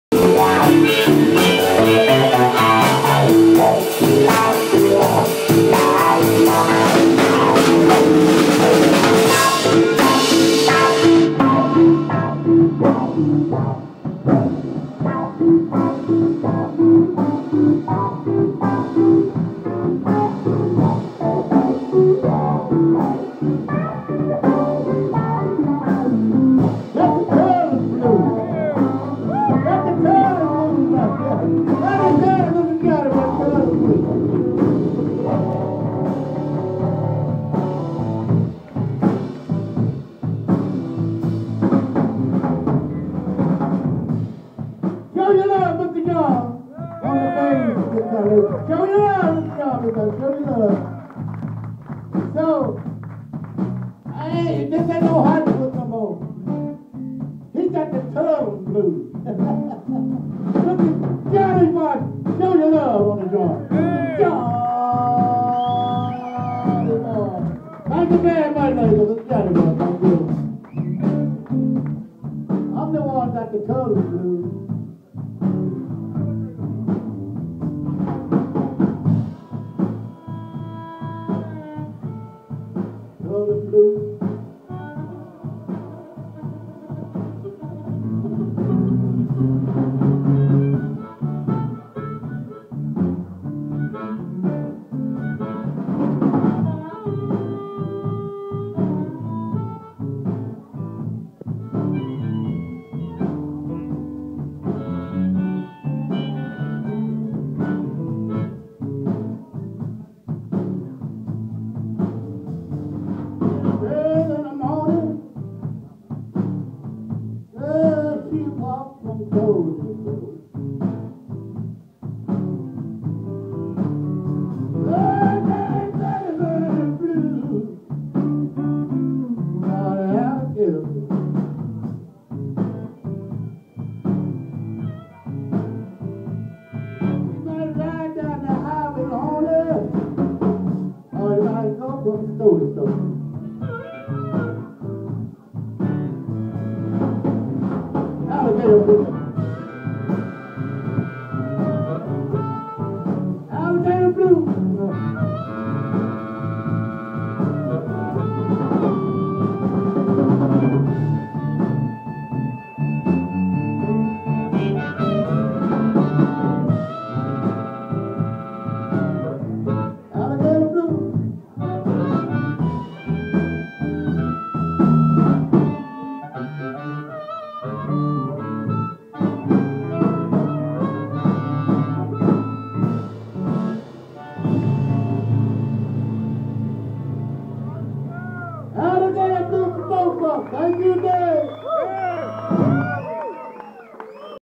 Boo Boo Davis Whispers 2
Boo Boo Davis live in muziekcafé Whispers te Assen. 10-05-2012.
Fantastische ondersteuning door 2 Nederlandse toppers.